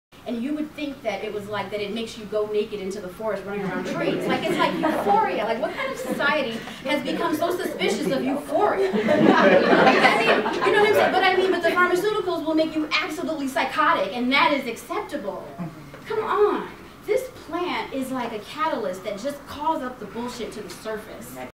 What's wrong with euphoria?!
Excerpt from a talk for the Chicago chapter of Women Grow.